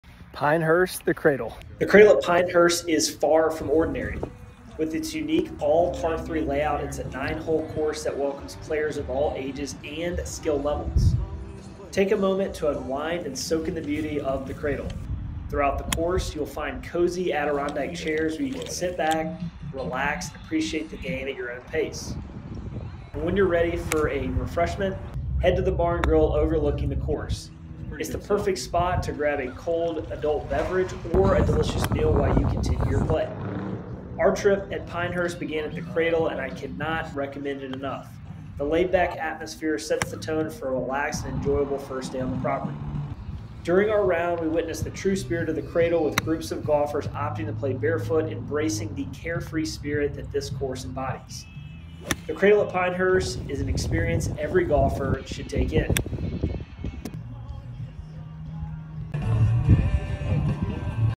A Day at The Cradle: Fun and Relaxation on Pinehurst's Short Course
The Cradle at Pinehurst is a golfing oasis that defies convention and invites players of all ages and skill levels to experience the joy of the game. This par-3 short course, nestled within the picturesque Pinehurst Golf Resort, offers a refreshing departure from traditional golf courses. With its meticulously designed nine holes, The Cradle presents a delightful challenge with undulating fairways, strategic bunkering, and imaginative green complexes. Beyond the game itself, The Cradle provides a haven for relaxation and connection, featuring inviting Adirondack chairs where players can pause and savor the breathtaking views. The bar and grill overlooking the course offers an ideal spot to recharge and indulge in delicious food and refreshing drinks. Whether you're a seasoned golfer or a newcomer to the sport, The Cradle at Pinehurst promises an unforgettable golfing experience filled with laughter, camaraderie, and cherished memories.

For more information about The Cradle, check out the videos posted on the social channels below.